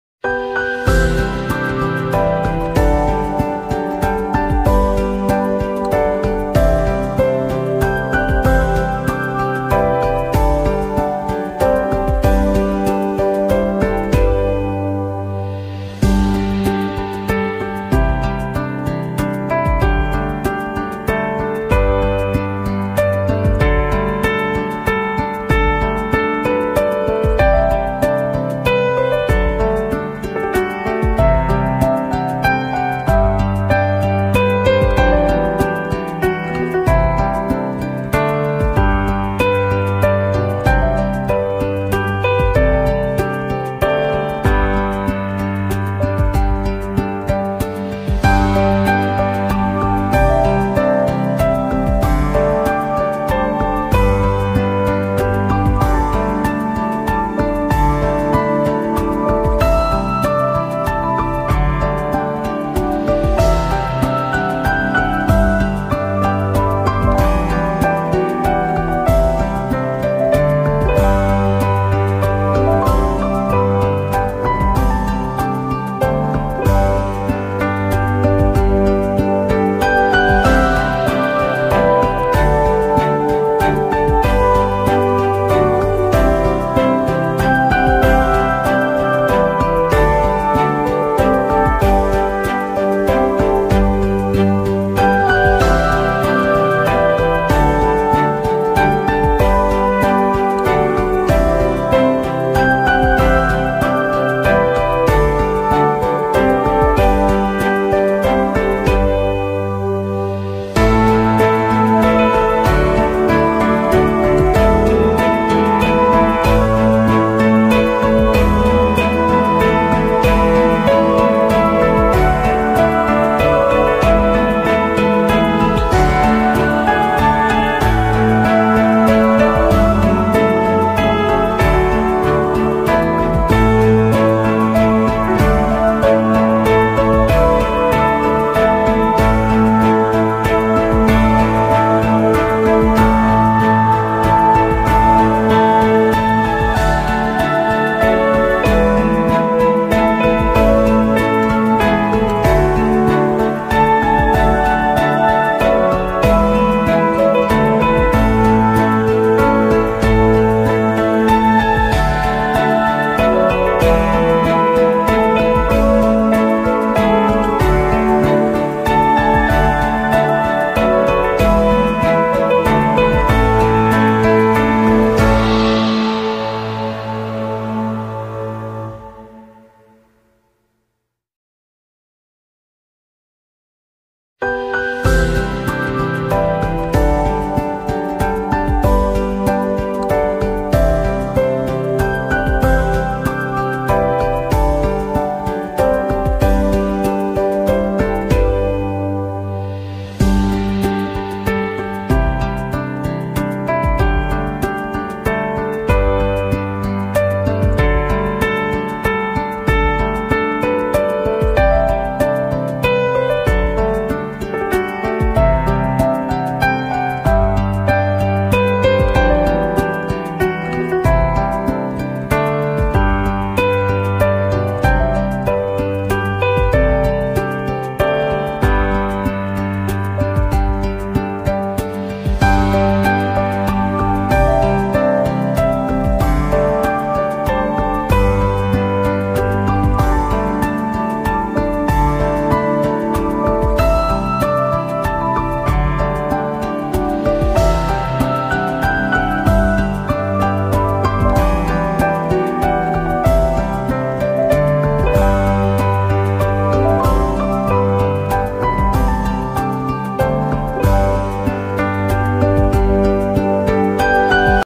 How To Make paper Sunflower
Paper craft,  paper craft flower,  design paper as bountiful craft, step by step process  of paper       sunflower